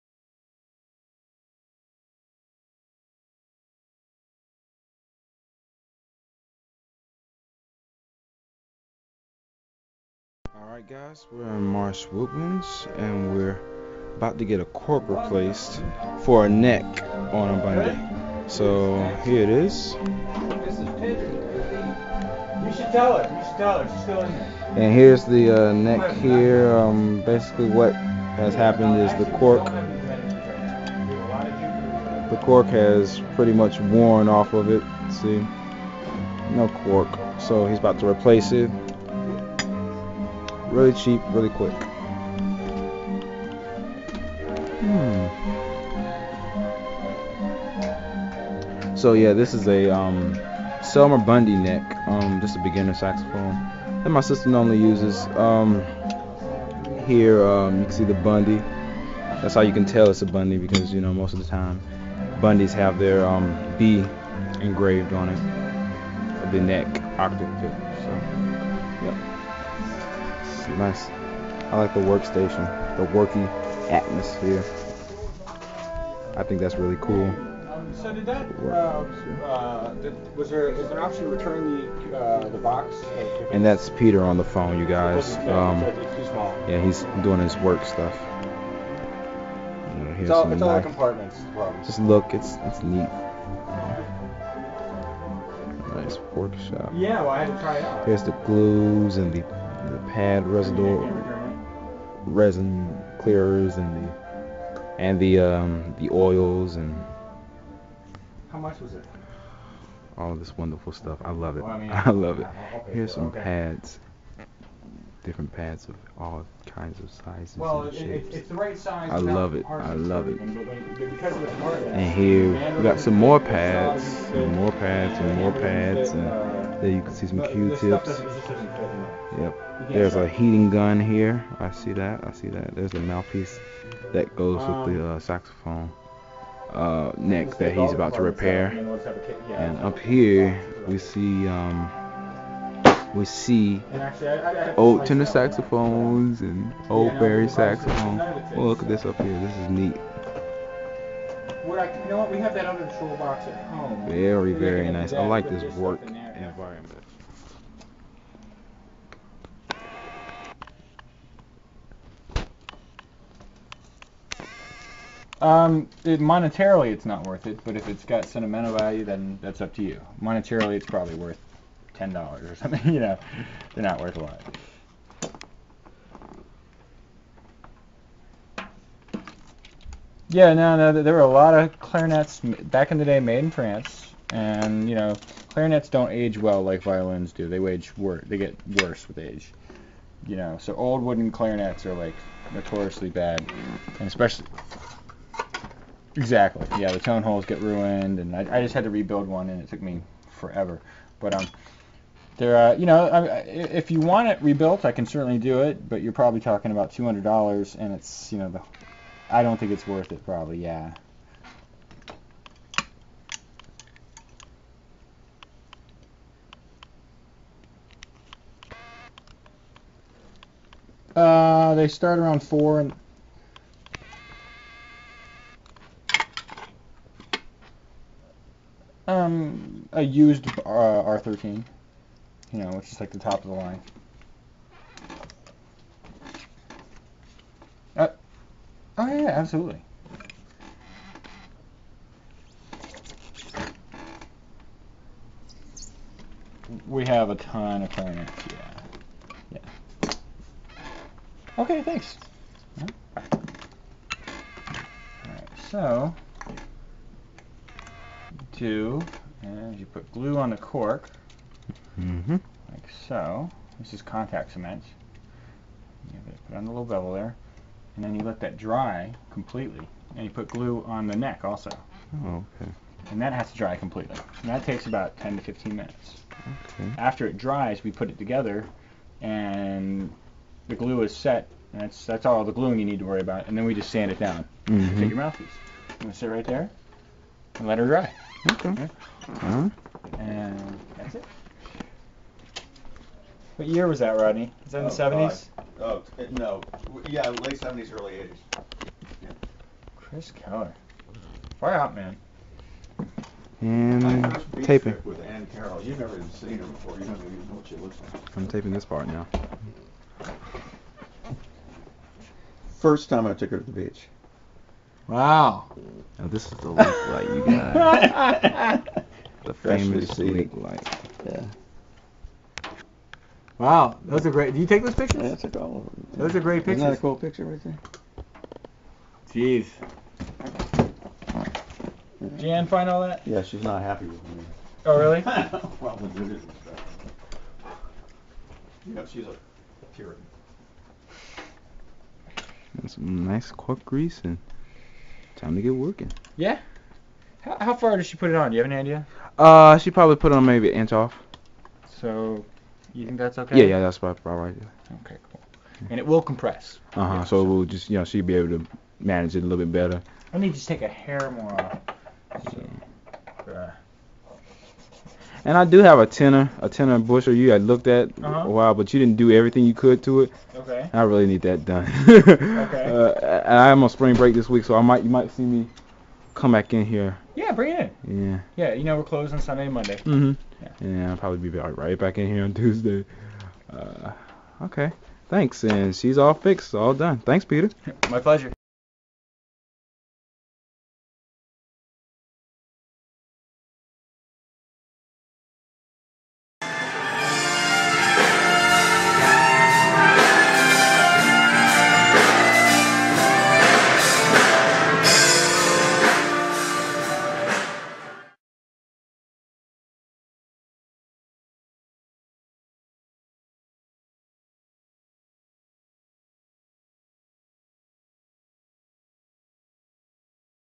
Project Sax Episode 32: Peter, "King Of The Sax Rings" (Sax Cork Fix)
Peter is my #1 sax fixer, and he fixes my sisters cork up really nice!!
Cheap and really quick